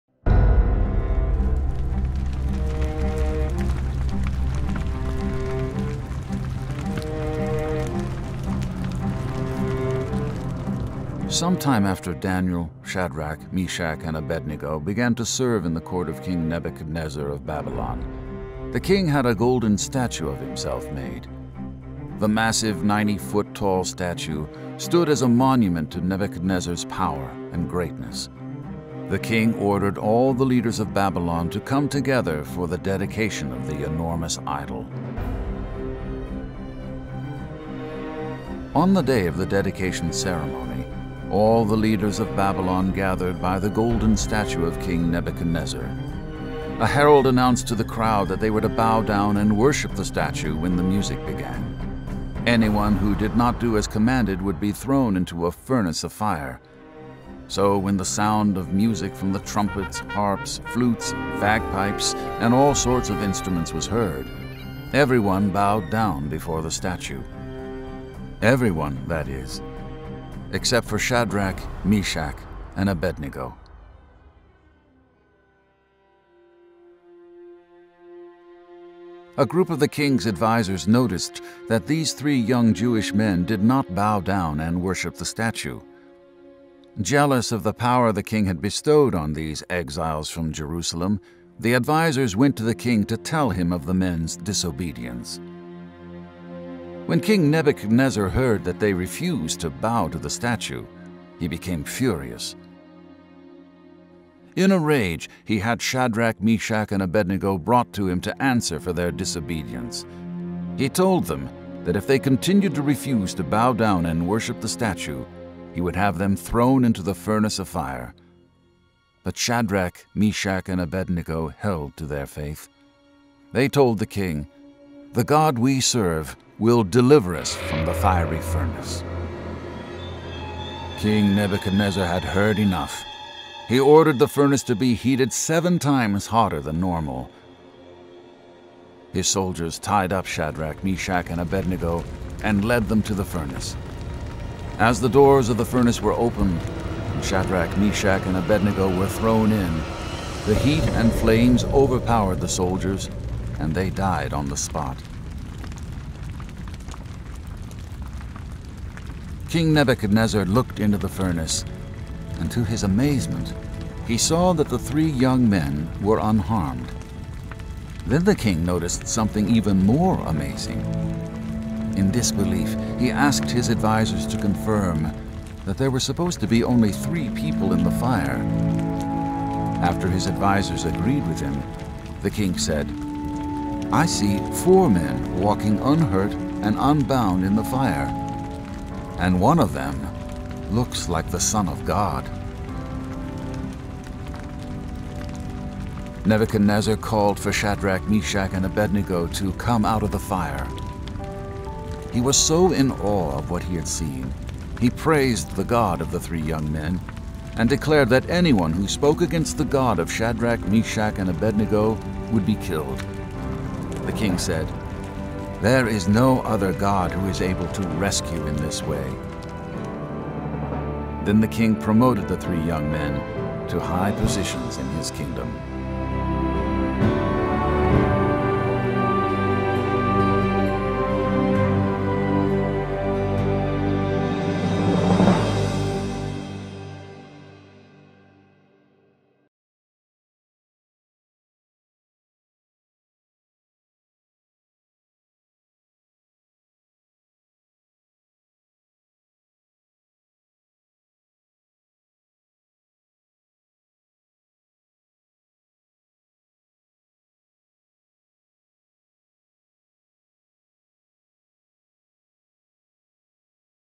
Shadrach, Meshach, and Abednego: The Furnace of Fire - Daniel 3 Bible Story | Sharefaith Kids
The Daniel 3 Fiery Fiery Furnace Kids Bible Video is an incredible story of courage, faith, and God’s mighty miracle-working power. After King Nebuchadnezzar commanded everyone in Babylon to bow down before his golden statue 3 young men named Shadrach, Meshach and Abednego refused. Their choices seamed like certain death but God miraculously met them in the fire! With award-winning artwork, amazing animation, and bold storytelling this Daniel 3 Bible Video for Kids is a must-have Sunday School resource.

Music and sound effects purchased and licensed from musicloops.com and pond5.com